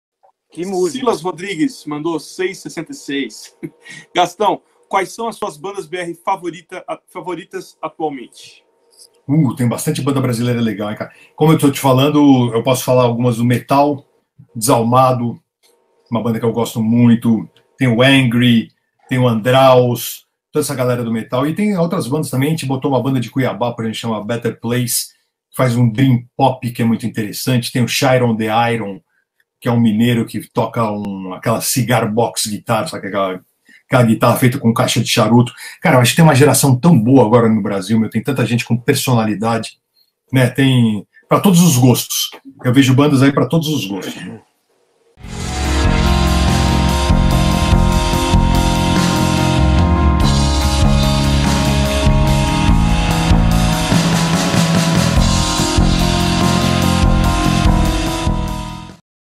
Better Place (Aquiles Priester entrevista Gastão Moreira) BETTER Cast #4
Aquiles Priester e Gilson Naspolini recebem Gastão Moreira na TV Maldita. Nesse Ep. Gastão Moreira (Kazagastão) indica bandas nacionais.